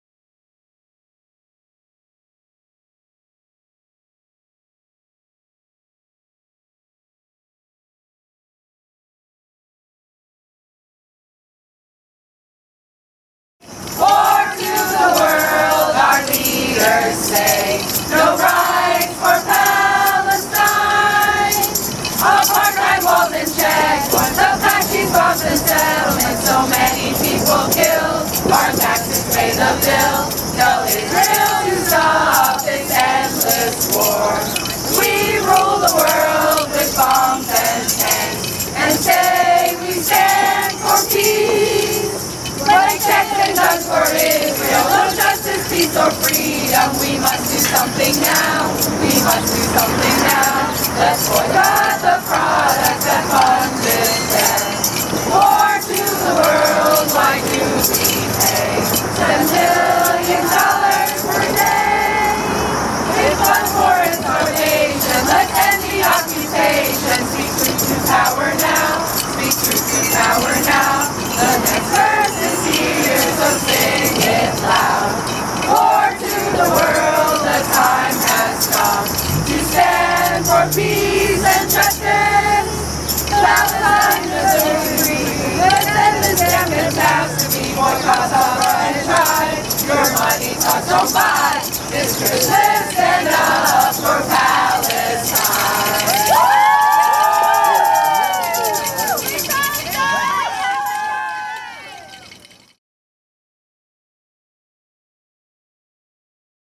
Philly BDS Caroling 2011
(Philadelphia, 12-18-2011) --Philly BDS, the local campaign for boycott, divestment and sanctions against Israel, brought holiday caroling to the sidewalks of West Philadelphia. The group of singers offered a spirited short program of Hanukkah and Christmas songs with lyrics revised to reflect support of Palestinian human rights.